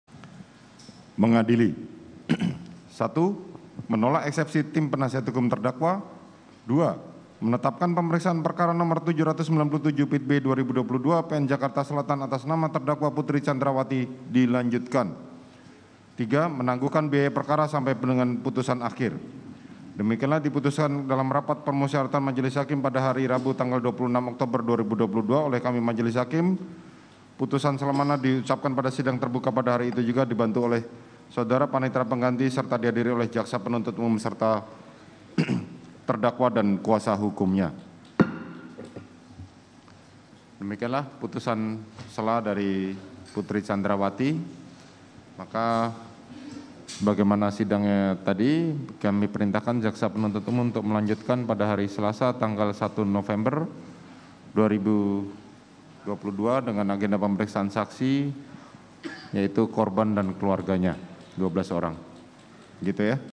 Bernasib Sama dengan Ferdy Sambo, Hakim Tolak Eksepsi Putri Candrawathi!
JAKARTA, KOMPAS.TV - Hari ini, Rabu 26 Oktober 2022, Pengadilan Negeri Jakarta Selatan kembali menggelar sidang dengan agenda pembacaan putusan sela untuk empat terdakwa yaitu Ferdy Sambo, Putri Candrawathi, Ricky Rizal, dan Kuat Ma'ruf.

Usai mendengarkan eksepsi, hakim memutuskan untuk menolak eksepsi Putri Candrawathi.

Sebelumnya, hakim juga memutuskan menolak eksepsi dari terdakwa Ferdy Sambo.